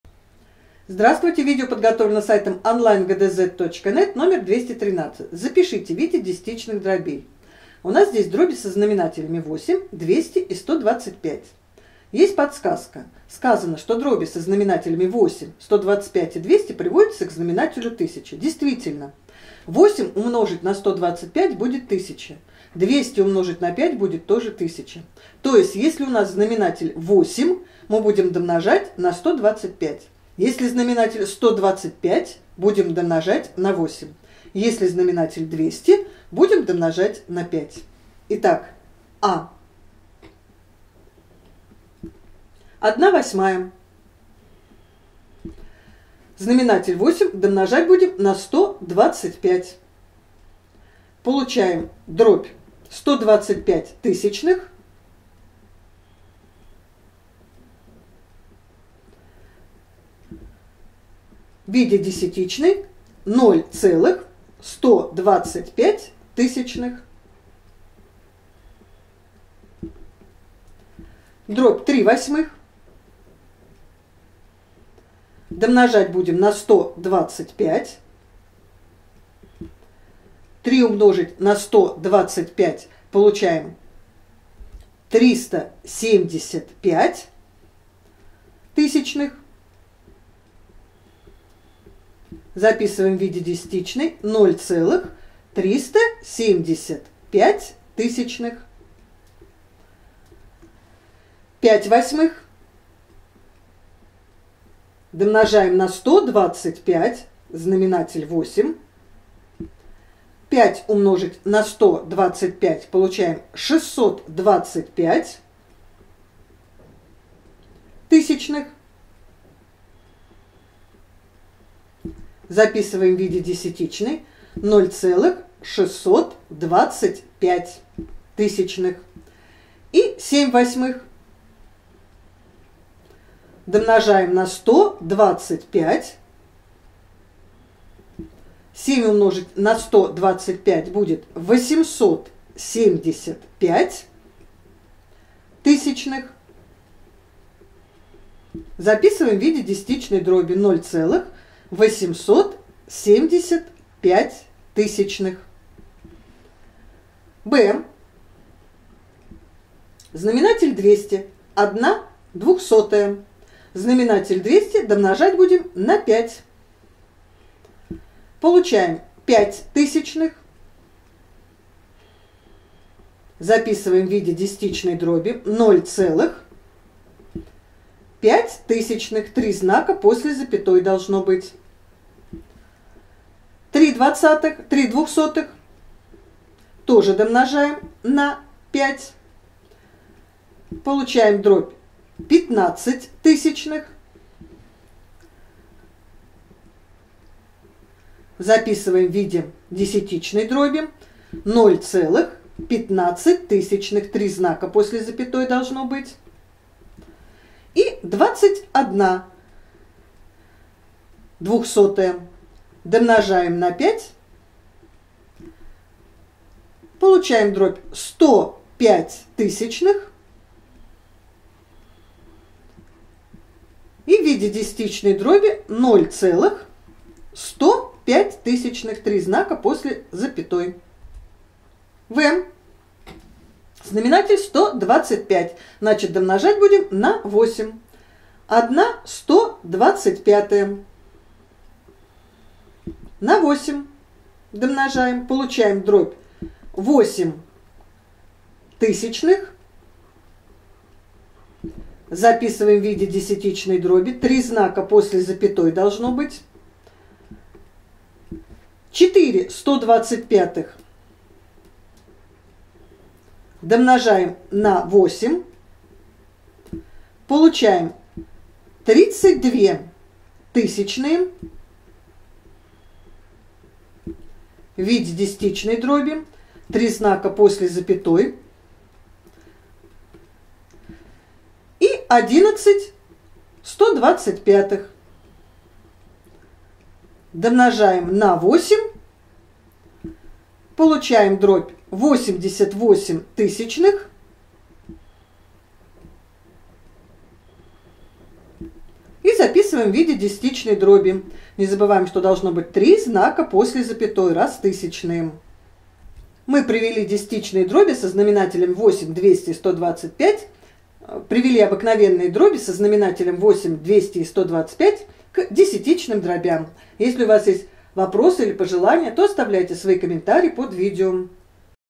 Задание №213 - ГДЗ по математике 6 класс (Дорофеев Г.В., Шарыгин И.Ф.)
Номер 213 – необходимо привести обыкновенные дроби со знаменателями 8, 125, 200 к знаменателю 1000 и записать получившиеся дроби в виде десятичных.